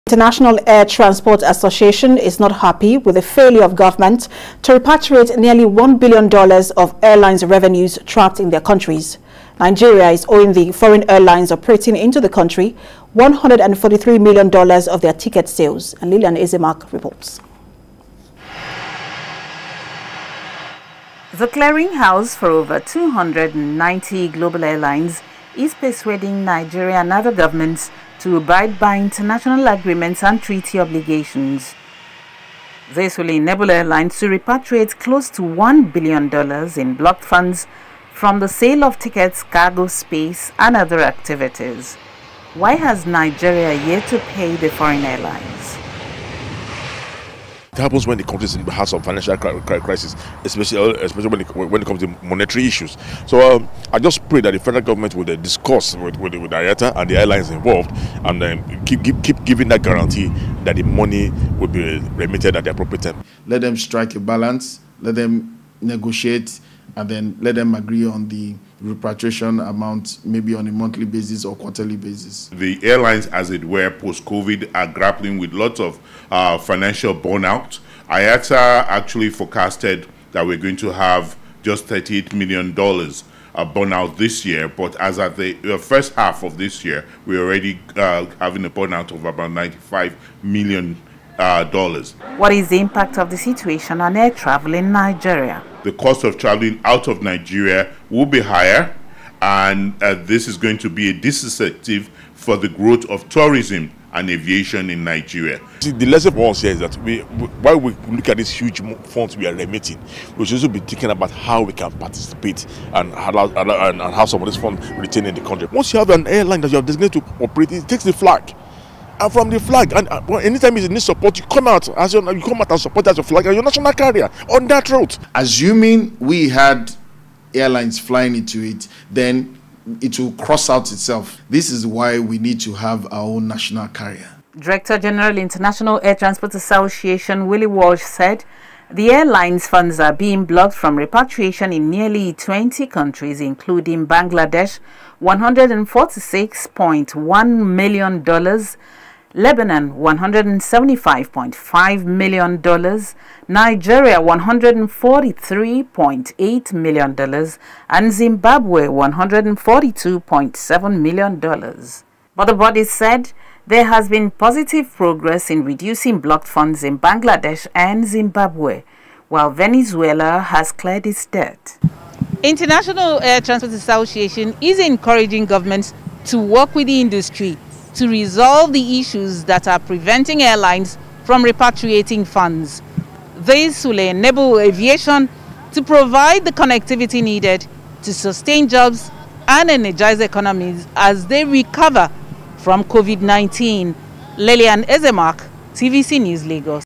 IATA Urges Nigeria, Others To Pay About $1bn
The International Air Transport Association is not happy with the failure of governments to repatriate nearly $1 billion of airlines’ revenues trapped in their countries.
Nigeria is owing the foreign airlines operating into the country $143 million of their tickets sales.